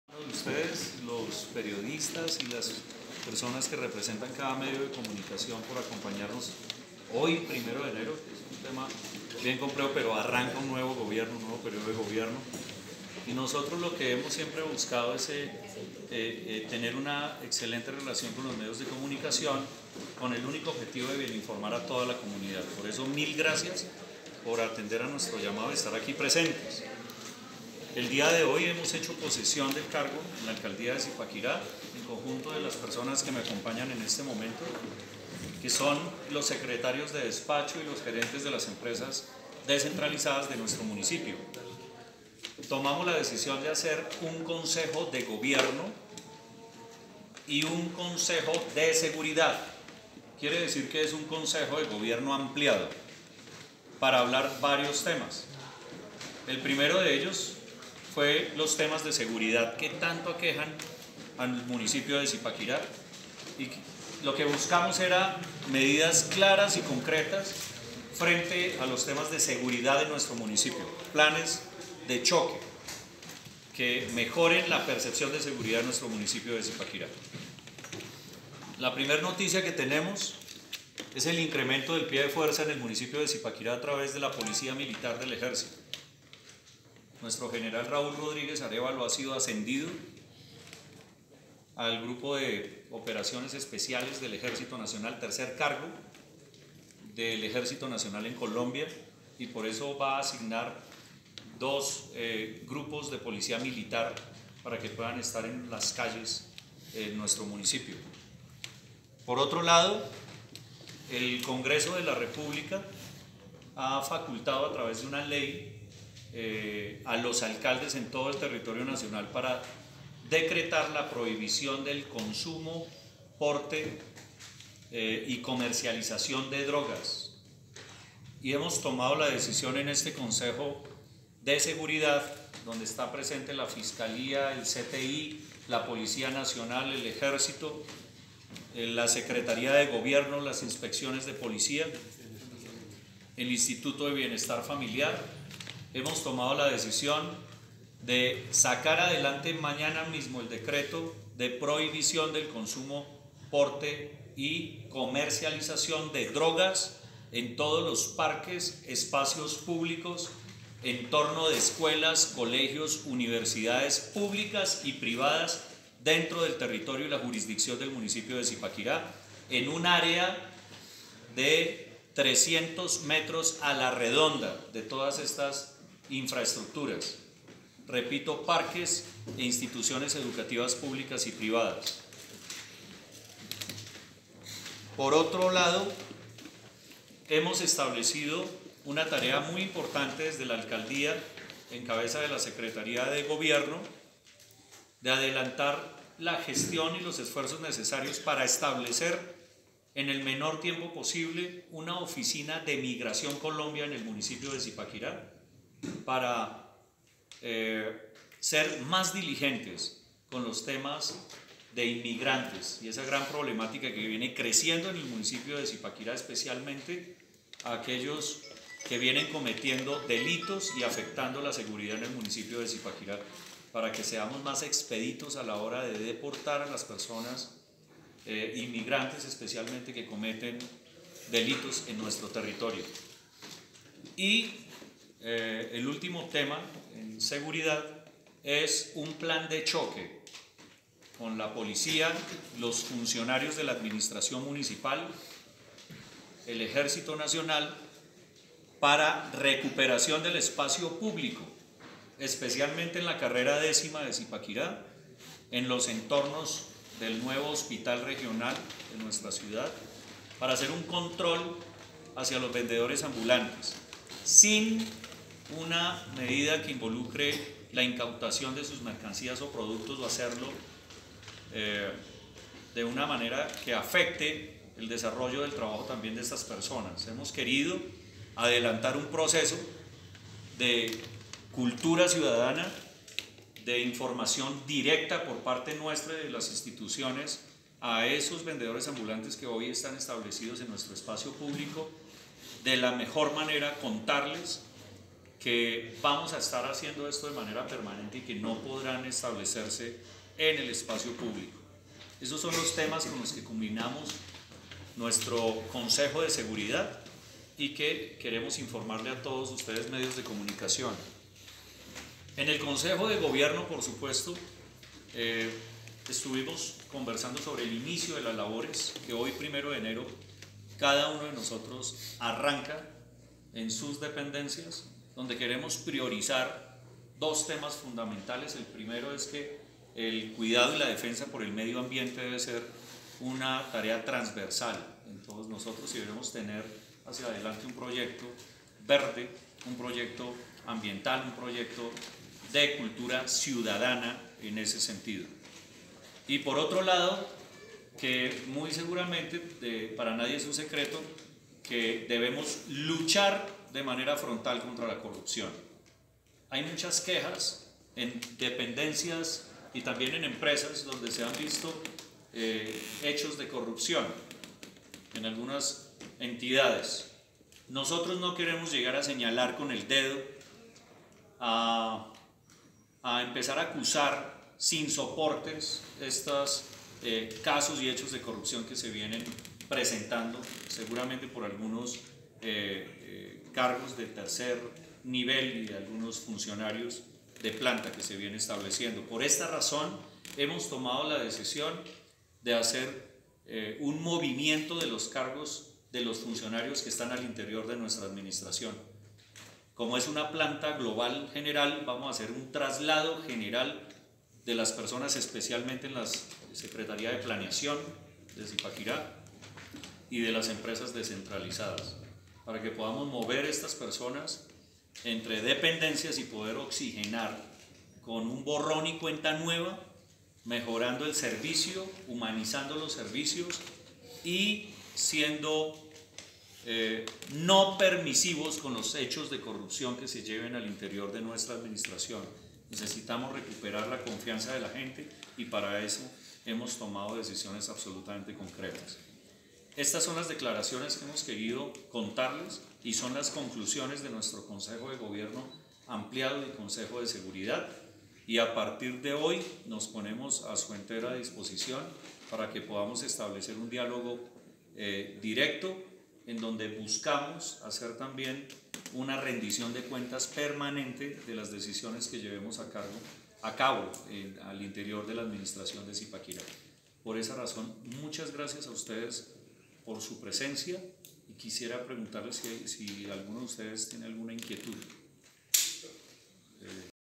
Aumenta el pie de fuerza en Zipaquirá
El primero de enero se ejecutan las primeras acciones relacionadas con la seguridad en la ciudad